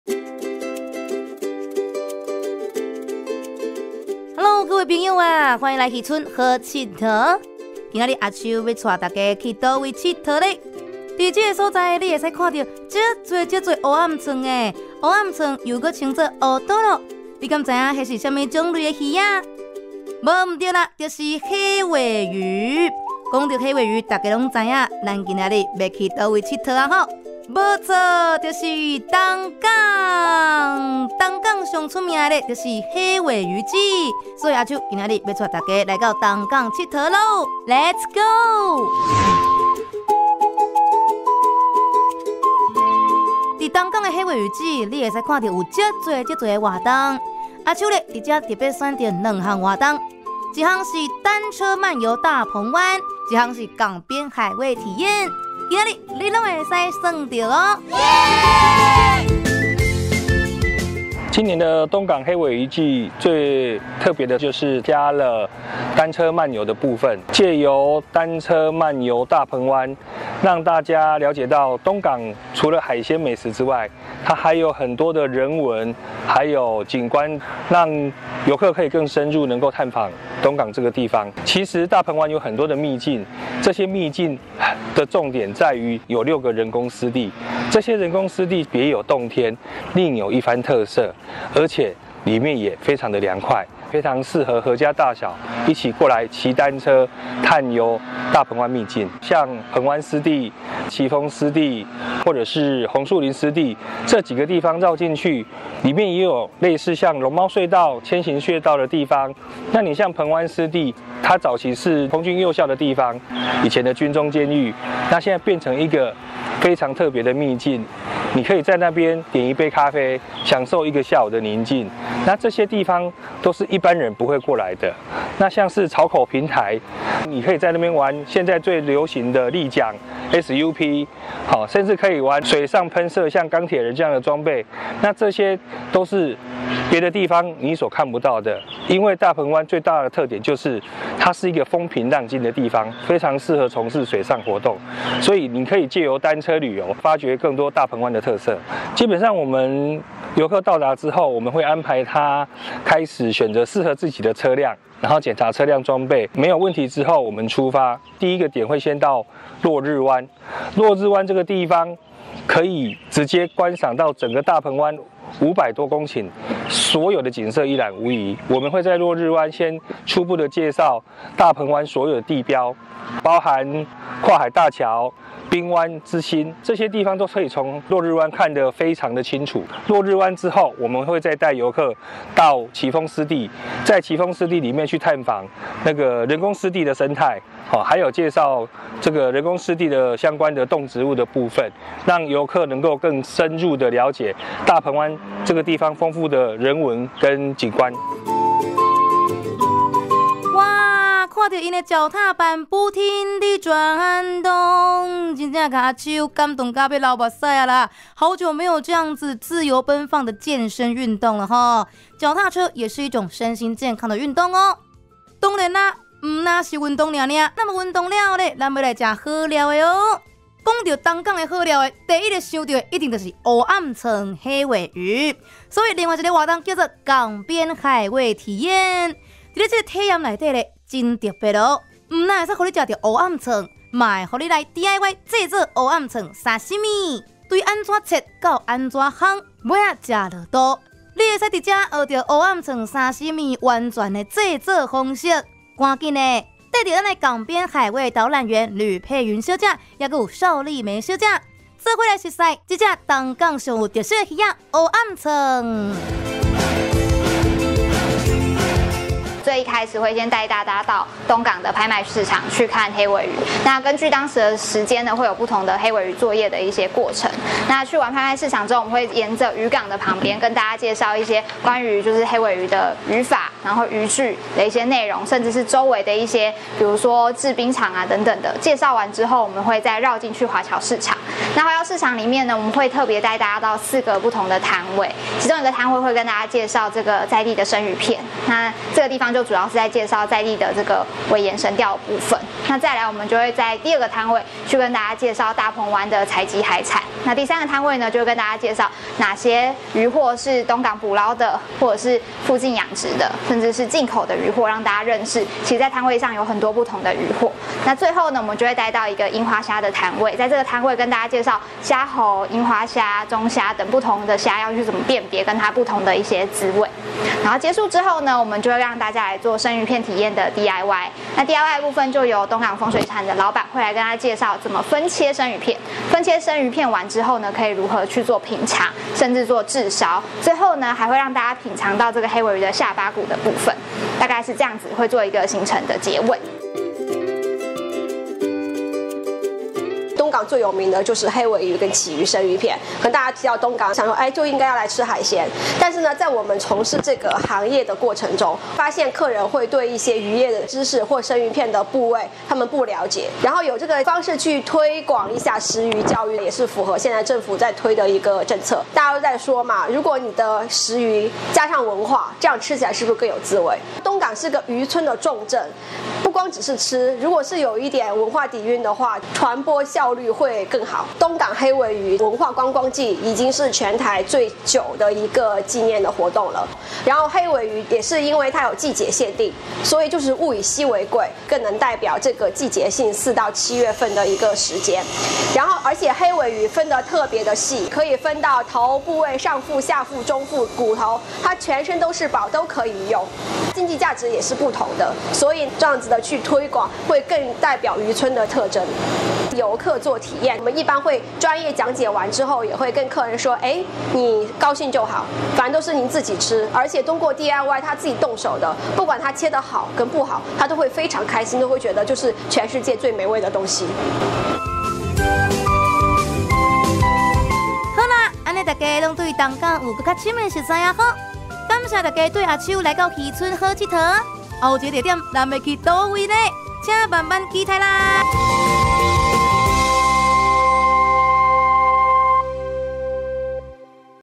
【漁村好𨑨迌】東港單車海味小旅行
近年為了因應自然環境改變，保育議題升溫等因素，活動融合在地特色、導入地方信仰、常民生活文化、休閒體驗活動與漁村小旅行等。